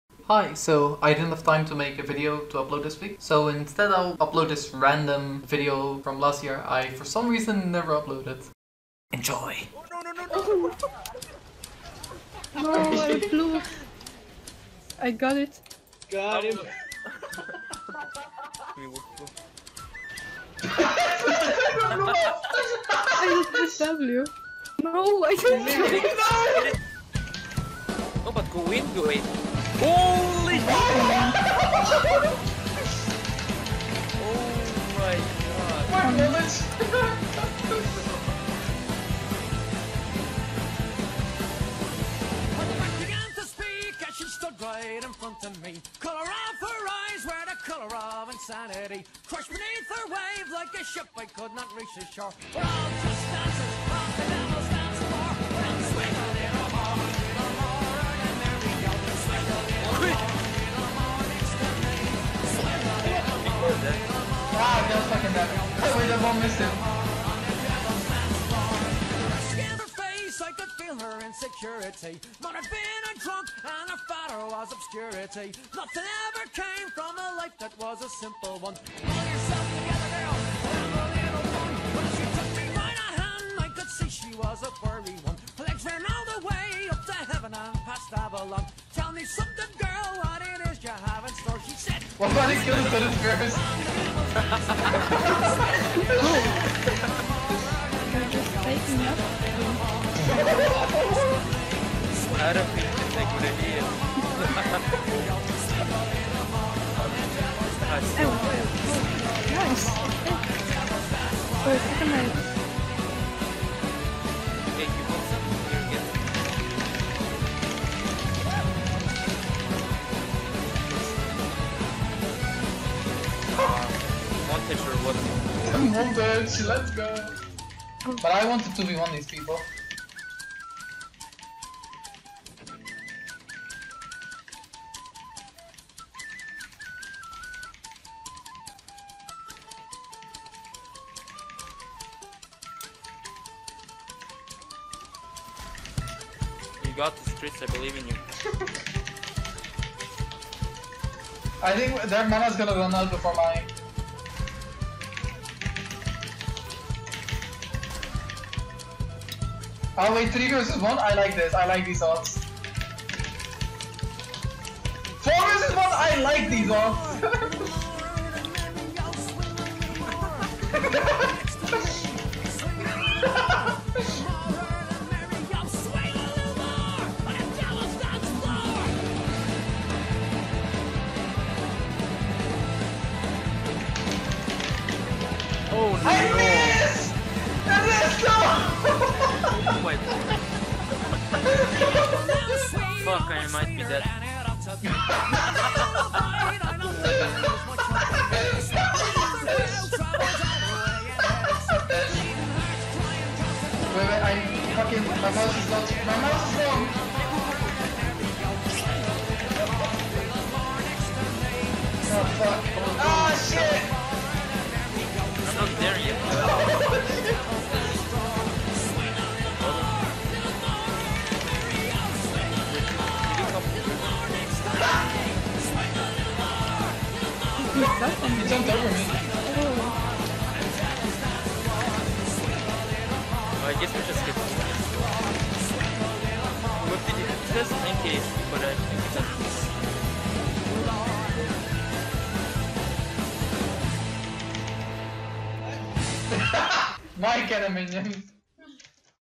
one for all shenanigans! (except it's from last year)
I made this last year but never uploaded it for some reason (thumbnail included)... so I'll just upload it now to keep my 1 video per week schedule... I was very busy :c

 It's very funny to see how my videos have changed since then! I'm considering making more videos with this style even though it gets lower views, just because they are so much fun to make xD

Link to discord for those who aren't in yet and wanna have some fun!